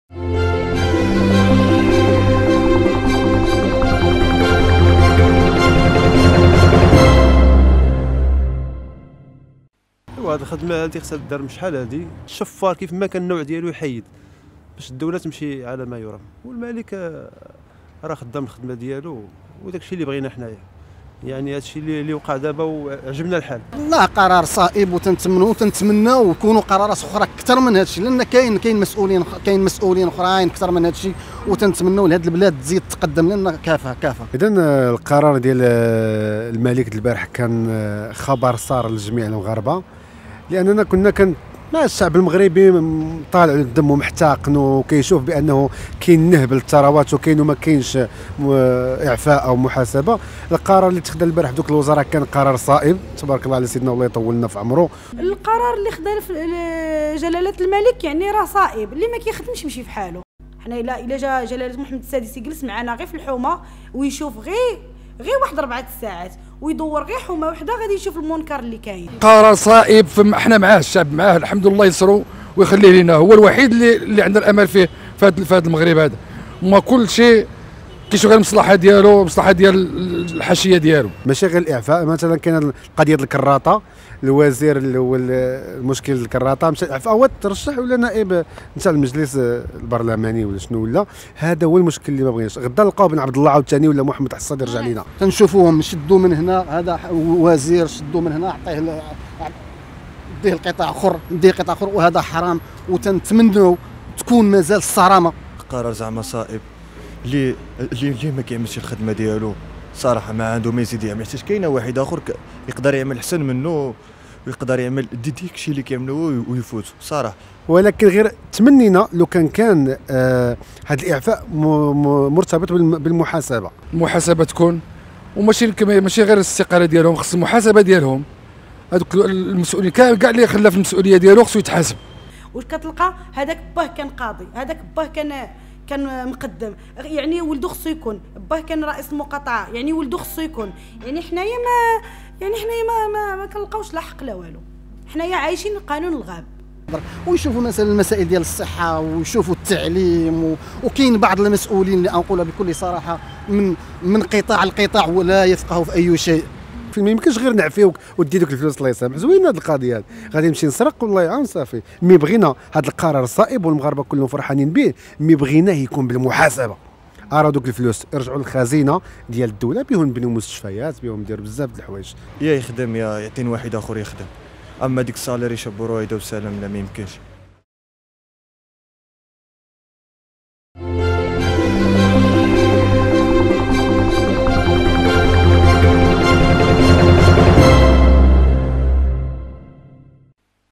هذا ما قاله ساكنة طنجة عن إعفاء الملك لوزراء بسبب الحسيمة
بالفيديو.. هذه آراء الشارع الطنجاوي حول إعفاء الملك لوزراء بسبب الحسيمة
طنجاوي
الزلزال السياسي الذي أحدثه الملك أمس، بقراره إعفاء وزراء من حكومة العثماني، وعدم رضاه عن وزراء سابقين، وقعوا أمامه اتفاقية مشروع "الحسيمة منارة المتوسط"، تلقاه الرأي العام بترحيب كبير، مؤكدين على ضرورة محاسبة هؤلاء الموقوفين وجميع المسؤولين الذين تبث إخلالهم بالمسؤوليات التي يتحملونها.
مكروفون موقع "طنجاوي" استقى عدد من آراء الشارع الطنجاويى حول هذه الإعفاءات، وما إذا كانت كافية أم لا ؟ فجاءت إجابتهم على الشكل التالي لنتابع: